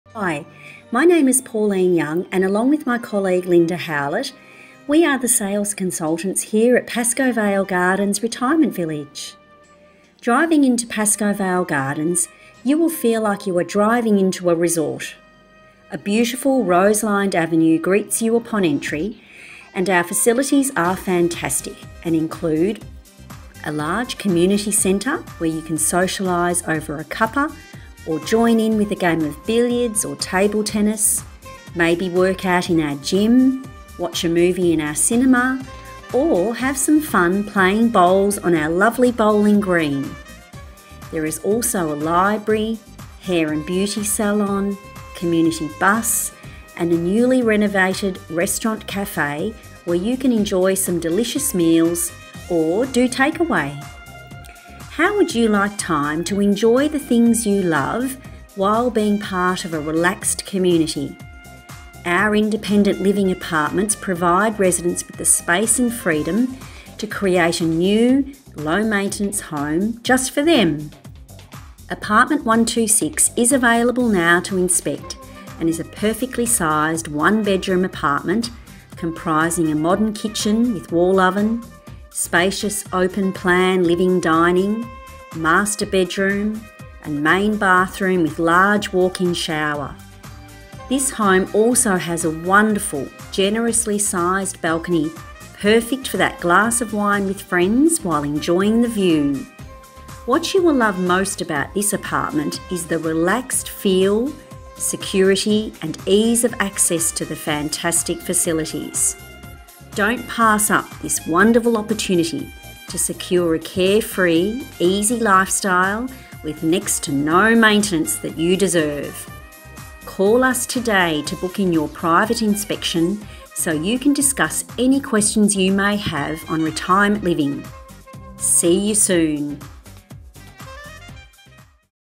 ILA 126 Pascoe Vale Gardens RV
Superbly and safely located just metres away from the fabulous Community ClubHouse within Pascoe Vale Gardens Retirement Village, this lovely, perfectly sized 1-bedroom Apartment is a delight! Crisp, clean lines compliment a homely and spacious open plan living/dining area which opens out onto a generously sized balcony, perfect for that glass of wine with friends while enjoying the view.